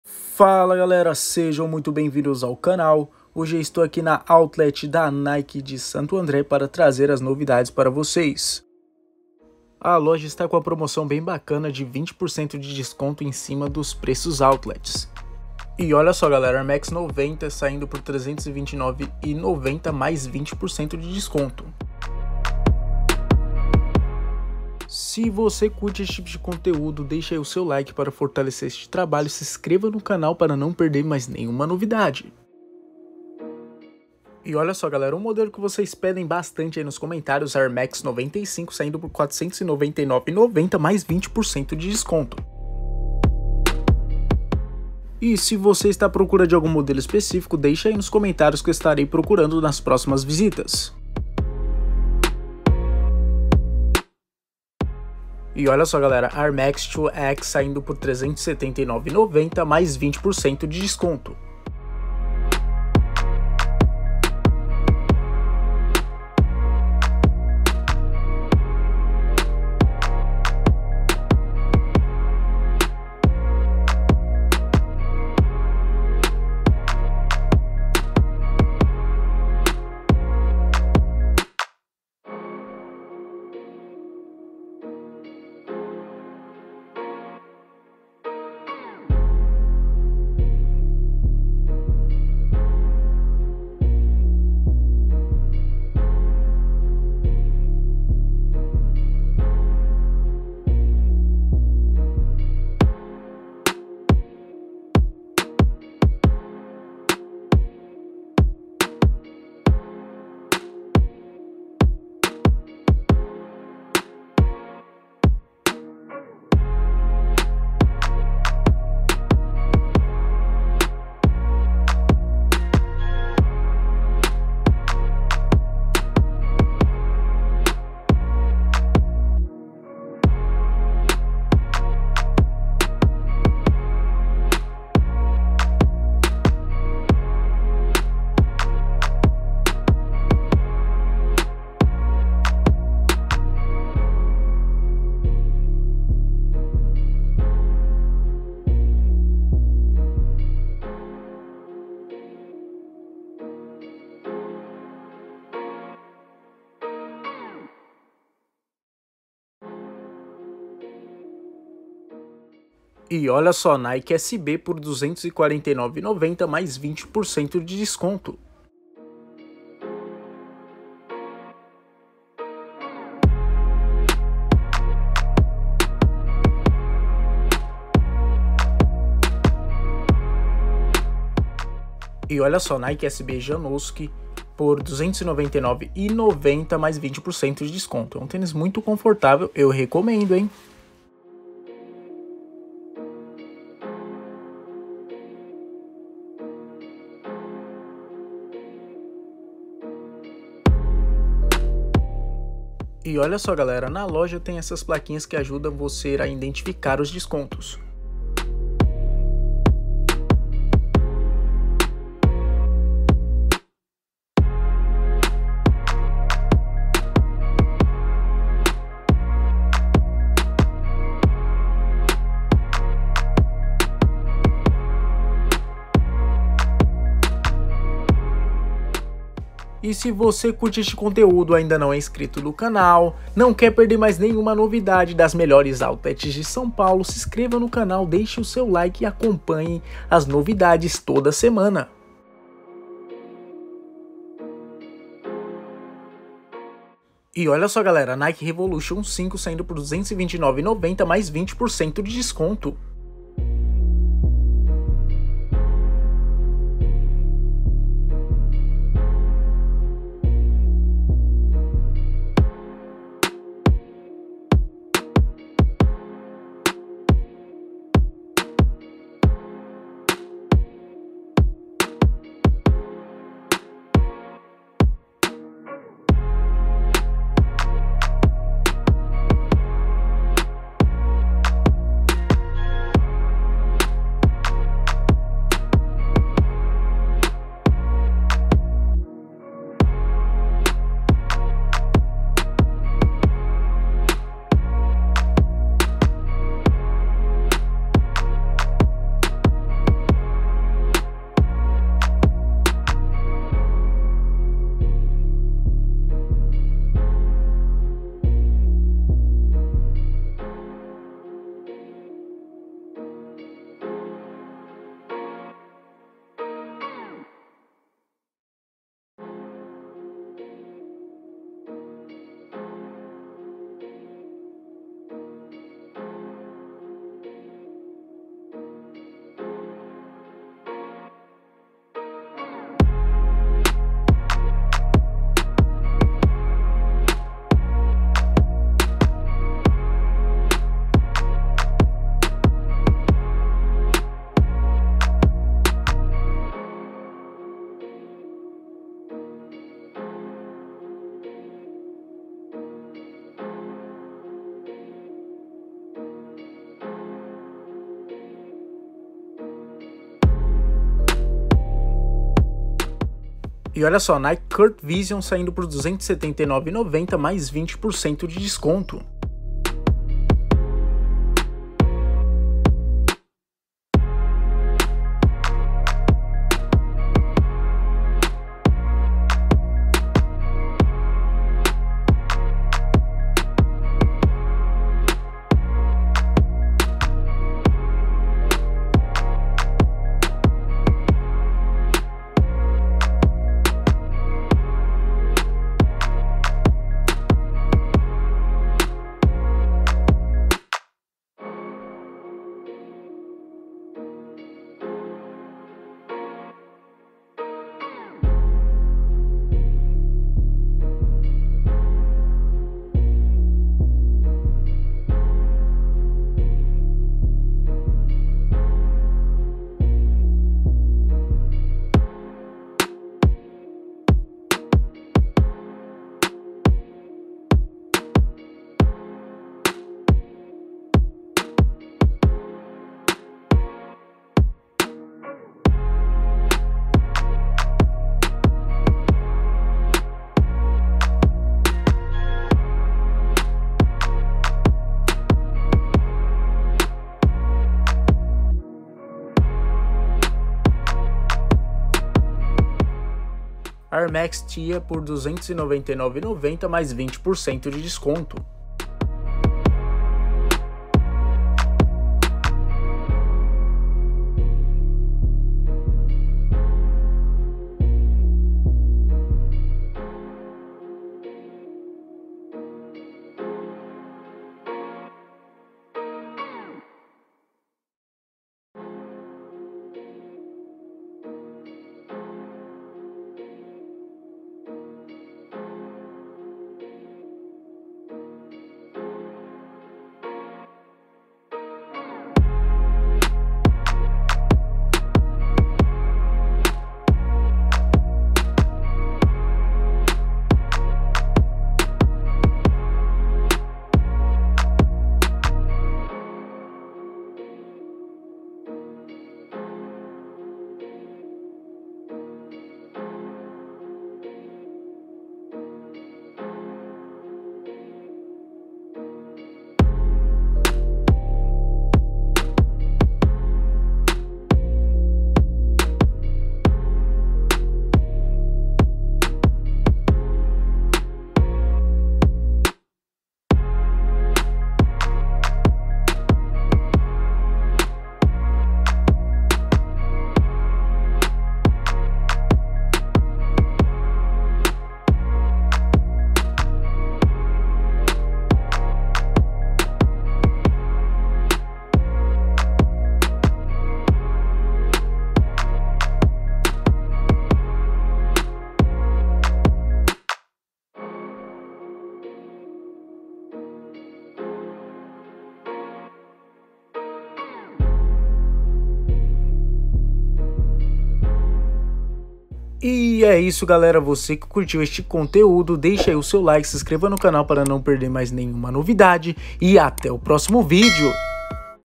OUTLET NIKE EM SANTO ANDRÉ - 20% DE DESCONTO - TÊNIS ORIGINAIS E BARATOS
Hoje temos mais uma novidade no canal!
Estarei mostrando tudo que encontrei na Outlet Nike de Santo André, a loja está aberta e seguindo os protocolos!

Aqui no canal tem novidade toda semana para ajuda-los a compararem os preços e realizarem a melhor compra!

✅ TEMA: OUTLET NIKE EM SANTO ANDRÉ - 20% DE DESCONTO - TÊNIS ORIGINAIS E BARATOS

🌐 Endereço: Praça do Carmo, Qd. 2, 13 - Centro, Santo André, SP.

📸 Acompanhe no Insta: @jon.de.araujo.ofc